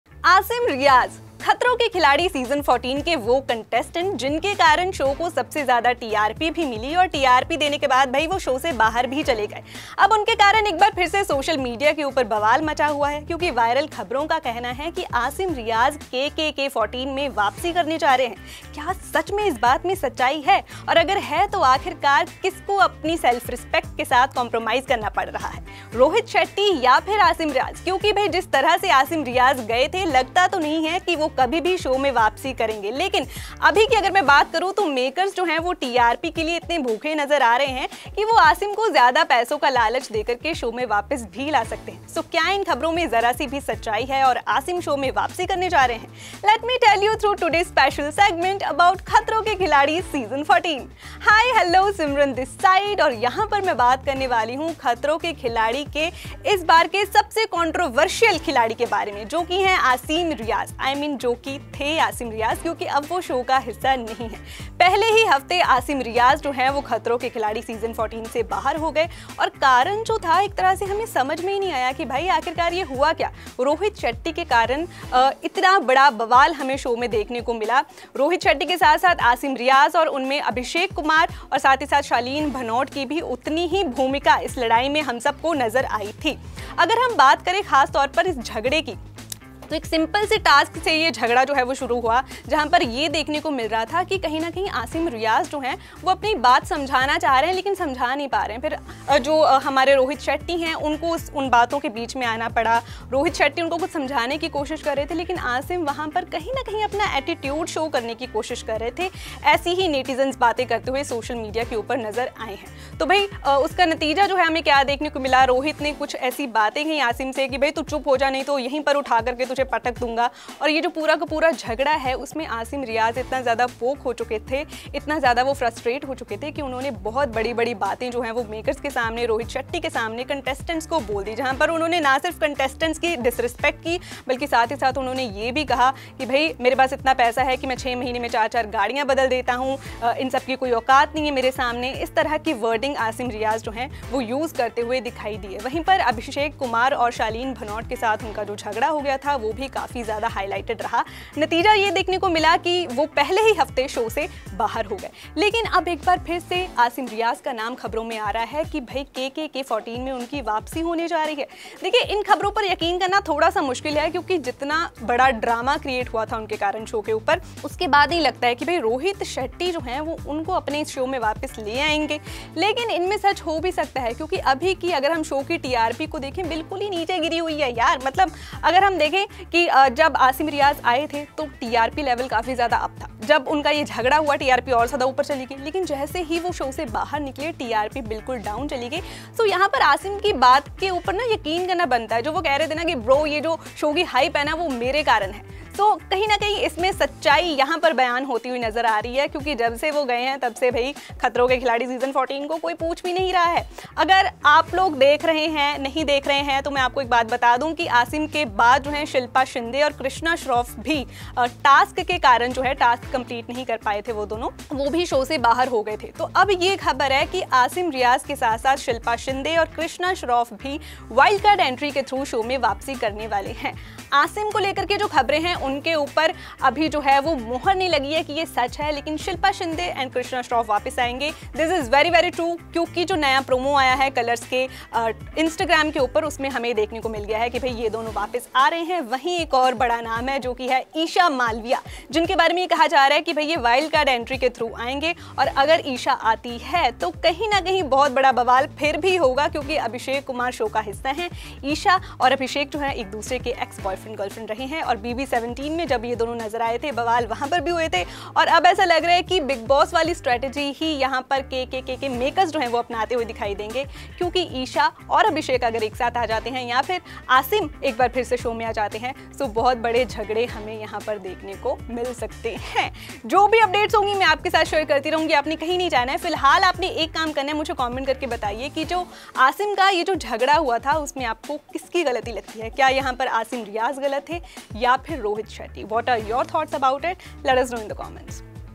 Asim Riyaz Back in KKK 14 | Khatron ke Khiladi 14 Wild Card Entries | अब शुरू होगा ख़तरनाक खेल 😈😈
In a surprising turn of events, reports suggest that Asim Riaz, who was previously eliminated from Khatron Ke Khiladi 14 following a massive altercation with Rohit Shetty, Shalin, and Abhishek Kumar, might be making a comeback on the show. Alongside him, Shilpa Shinde and Krishna Shroff are set to rejoin the season as new wild card contestants. Additionally, Isha Malvia is rumored to be making an appearance.